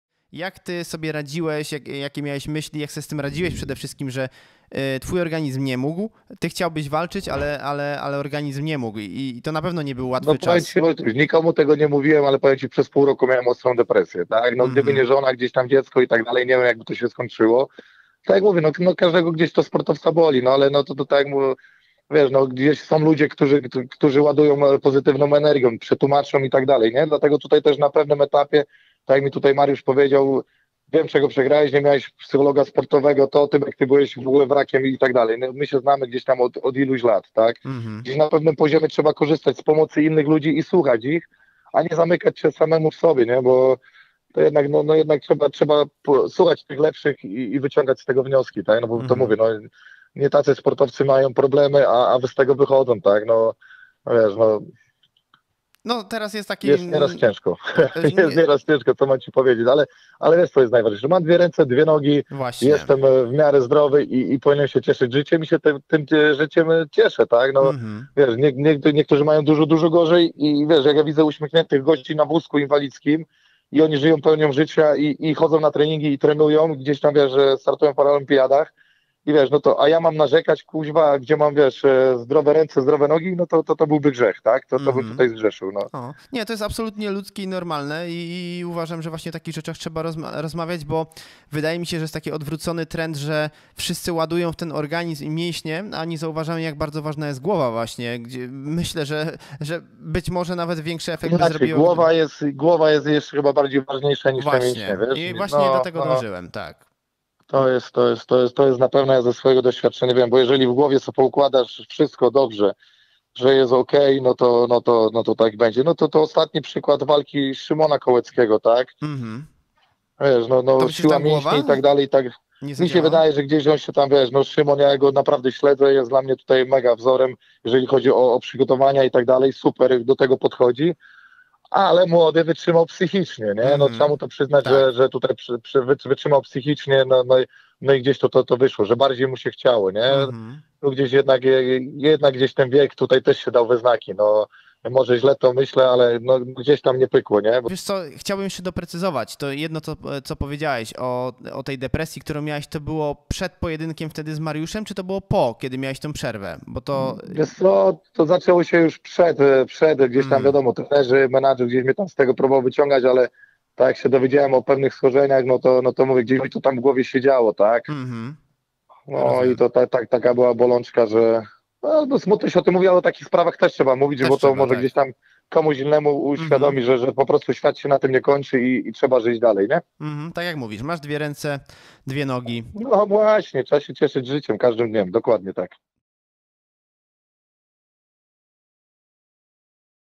Tyberiusz Kowalczyk pokonał ostrą depresję po walce z Pudzianowskim
Wyjątkowo smutne informacje ale w pozytywnym świetle, przedstawił nam w MMAnews Live #73 Tyberiusz Kowalczyk, który wspomniał o długiej przerwie i związanej z nią depresją, z jaką zmagał się swego czasu. Okazuje się, że w dużej mierze pomógł mu jego były rywal Mariusz Pudzianowski.